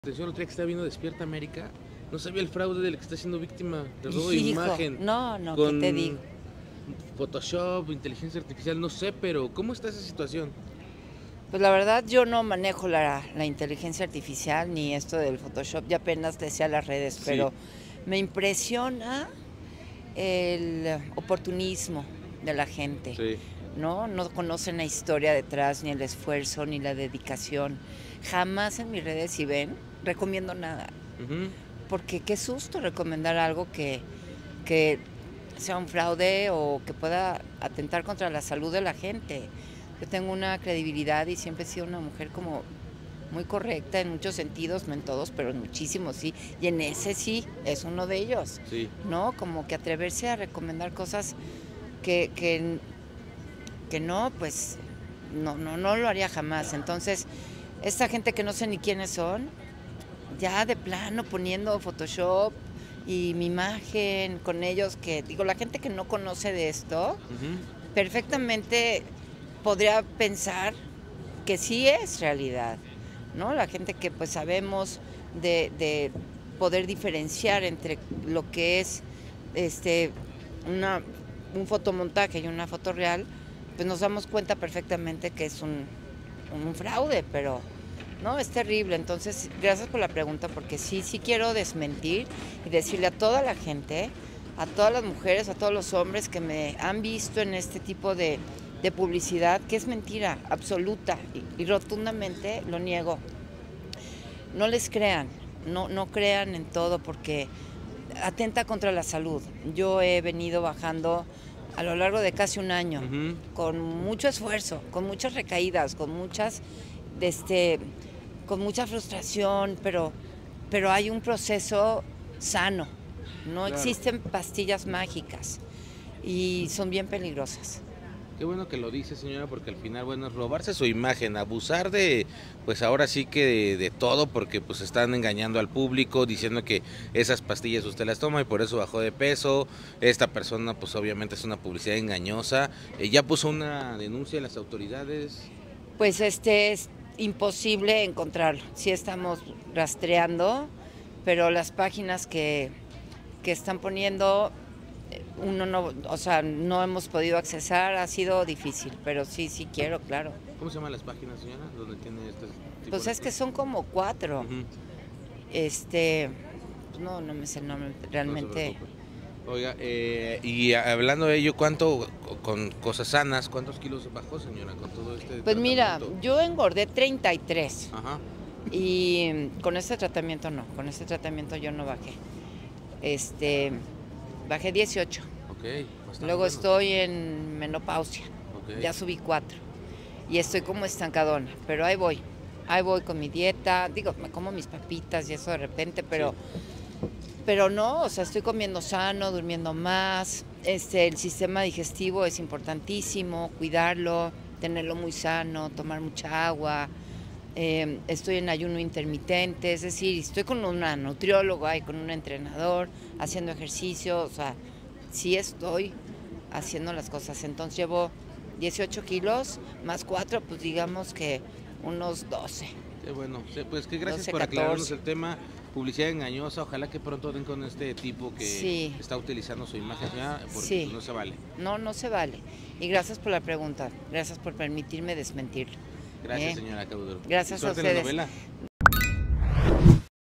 ¡ENGAÑAN A LA GENTE USANDO SU IMAGEN! Eugenia Cauduro no promociona productos para bajar de peso
Eugenia Cauduro hablo de las personas que han estado utilizando su imagen para promocionar productos milagro para bajar de peso y dijo que ella jamas haría eso y alerta a la gente a no creer en este engaño.

SÍGUEME